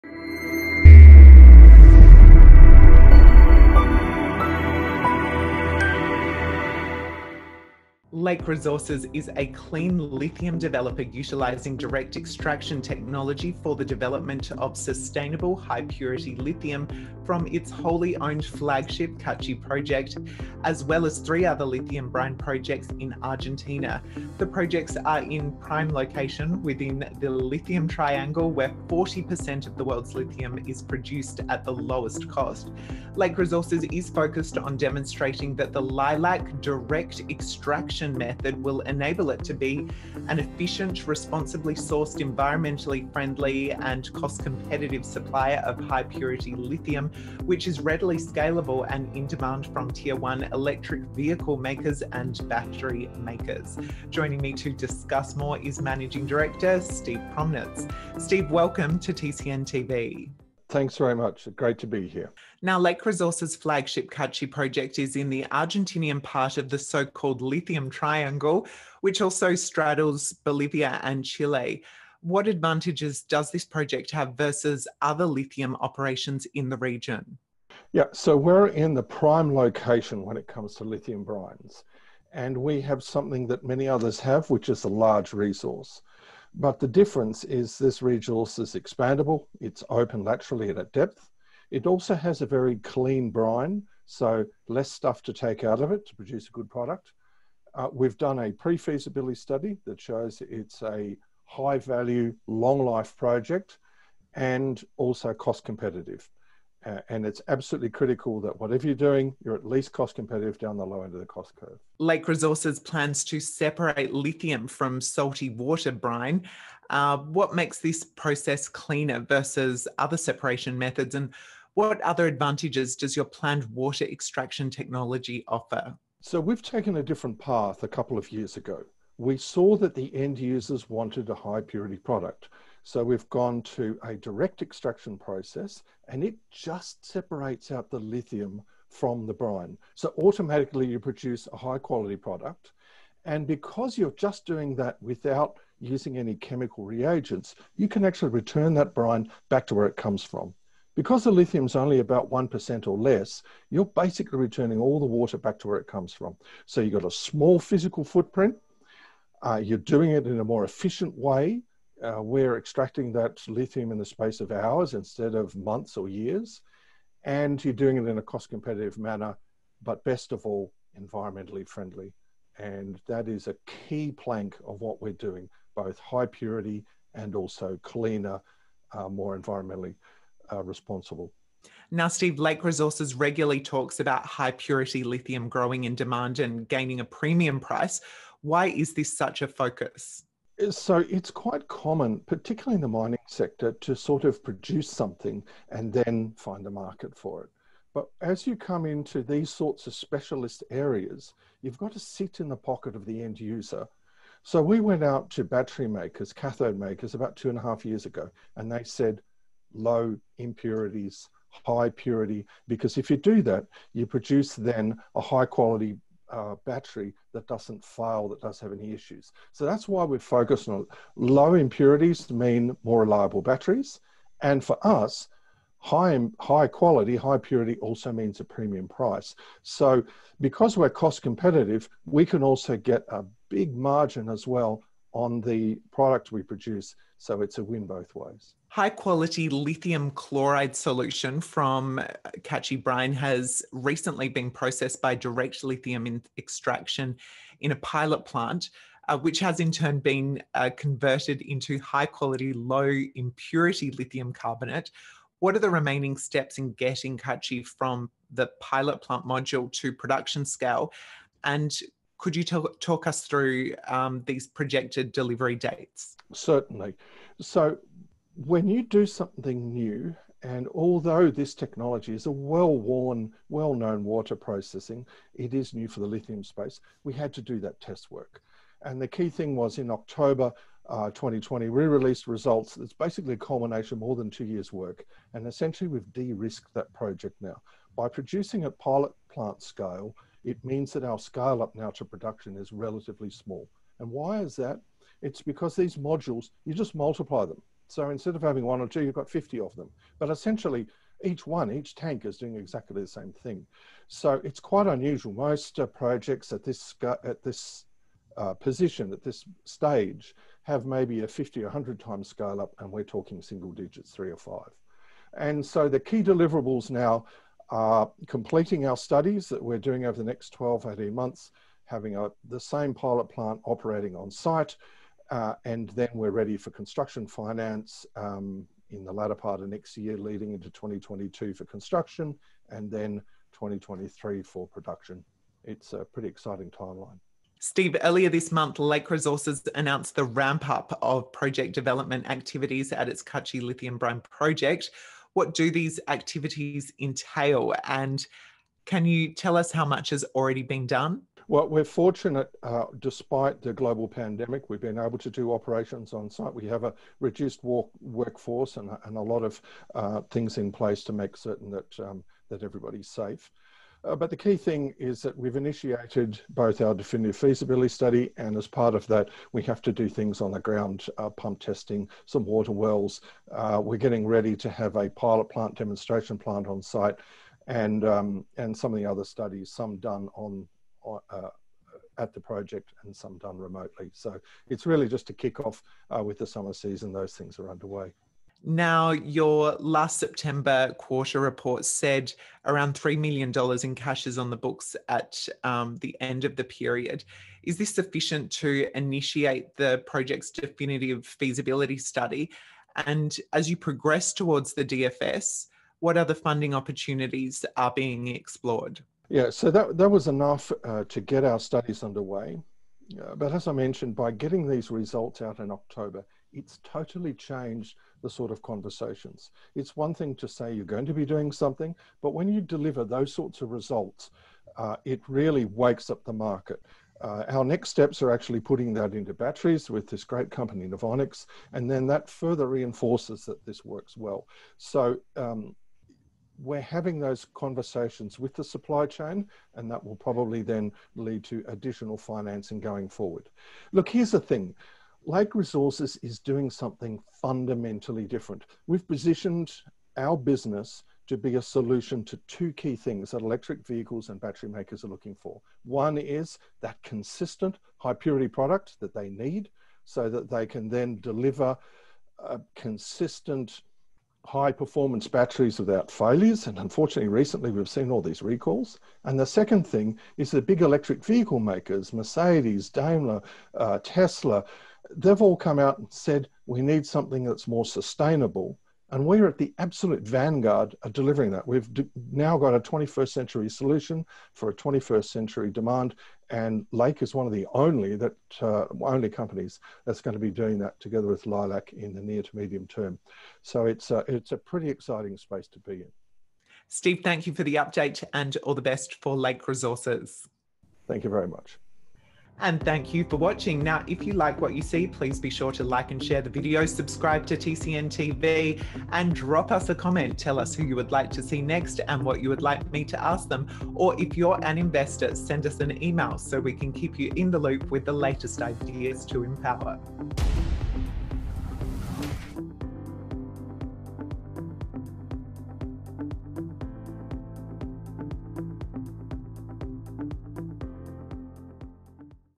Lake Resources NL (ASX:LKE): Progressing development of its ‘clean’ lithium project & process
November 2020 - Lake Resources NL (ASX:LKE, FRA:LK1, OTC:LLKKF) is a clean lithium developer utilising direct extraction technology for the development of sustainable, high purity lithium from its 100%-owned flagship Kachi Project, as well as three other lithium brine projects in Argentina. The projects are in a prime location within the Lithium Triangle, where 40% of the world’s lithium is produced at the lowest cost. Lake Resources is focused on demonstrating that the Lilac direct extraction method will enable it to be an efficient, responsibly-sourced, environmentally friendly and cost competitive supplier of high-purity lithium, which is readily scalable, and in demand from Tier 1 electric vehicle makers and battery makers. Managing Director Steve Promnitz speaks with The Capital Network’s Lel Smits.

Chapters
0:00-1:02 - Introduction
1:02-1:51 - Kachi Project metrics
1:51-3:01- Water extraction technology
3:01-3:58 -  High Purity Lithium
3:58-5:37 - Pilot Plant Module to Production Scale
5:37-6:28 - Activities Underway at Kachi
6:28-8:16 - Cash in hand for Kachi DFS
8:16-8:52- Conclusion

Questions
1. Kachi Project metrics (life; cost competitiveness; high purity and margin)
Lake Resources’ flagship Kachi Project is in the Argentinean part of the so-called lithium triangle, which also straddles Bolivia and Chile. What advantages does this project have versus other lithium operations in this region?

2. Water extraction technology - clean and price competitive with ESG benefit
Lake Resources plans to separate lithium from salty water (brine). What makes this process cleaner versus other separation methods and what other advantages does your planned water extraction technology offer?

3. High Purity Lithium – premium product; premium price
Lake Resources regularly talks about high purity lithium growing in demand and gaining a premium price. Why is this such a focus?

4. Progress in moving Kachi project from Pilot Plant Module to Production Scale
High quality lithium chloride solution from Kachi brine has recently been processed by direct lithium extraction in a pilot plant, which has in turn been converted into high quality, low impurity lithium carbonate. What are the remaining steps in getting Kachi from pilot plant module to production scale? What are their projected delivery dates?

5. Activities Underway at Kachi
Earlier this month, Lake Resources announced the ramp up of project development activities at its Kachi Lithium Brine Project. What do these activities entail and how much has been done already?

6. Cash in hand for Kachi DFS; discussions with ‘international investment funds’ to secure more funding
Lakes Resources’ September 2020 quarter activities and cash flow report said the circa $3m in cash on the books at period end was sufficient to initiate the Kachi Project’s Definitive Feasibility Study (DFS). As progress towards this DFS occurs, what other funding opportunities are being explored?

The opinions expressed in this video do not reflect the official position of MarketLit and do not constitute investment advice.